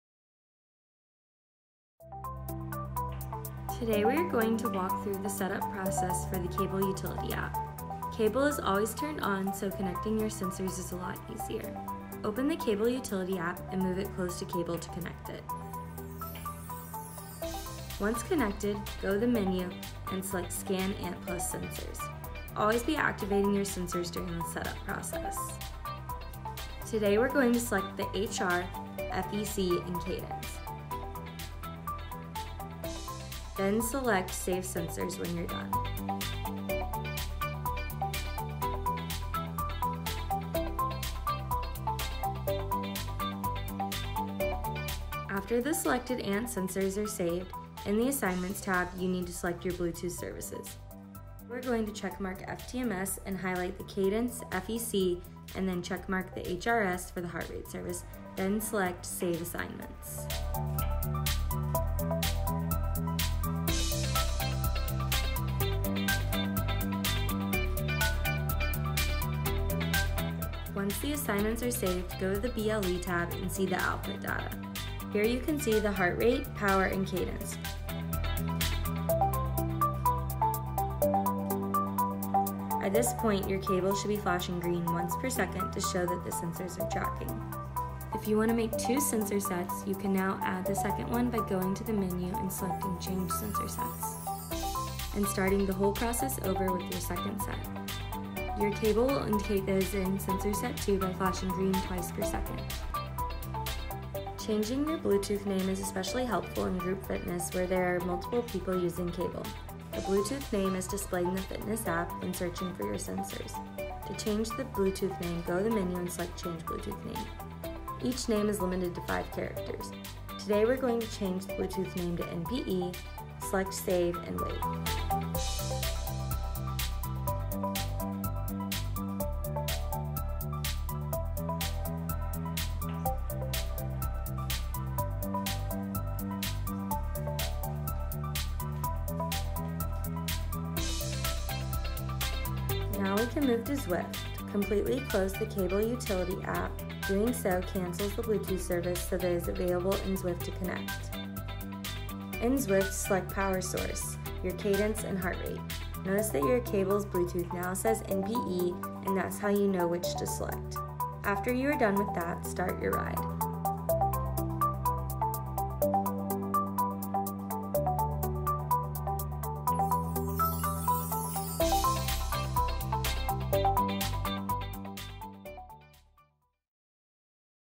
Connect CABLE to Zwift with Apple TV
Connecting Wahoo KICKR, Cadence sensor and Heart Rate Monitor to Zwift with Apple TV using CABLE (Connect ANT+ to Bluetooth).